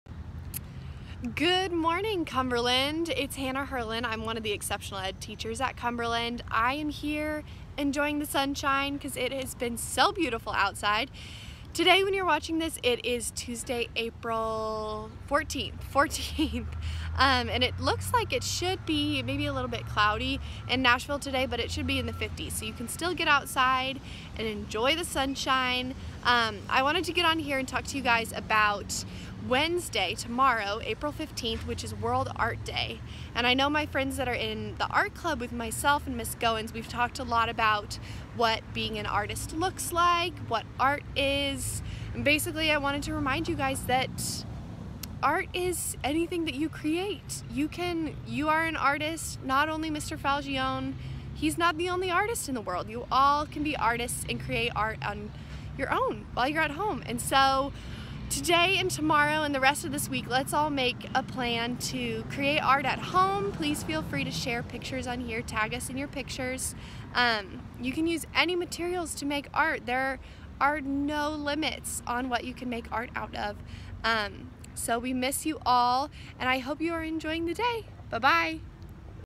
[Morning Meeting] World Art Day with Ms. Heurlin
Wednesday, April 15th, is World Art Day. Make some art this week and every week, Cumberland!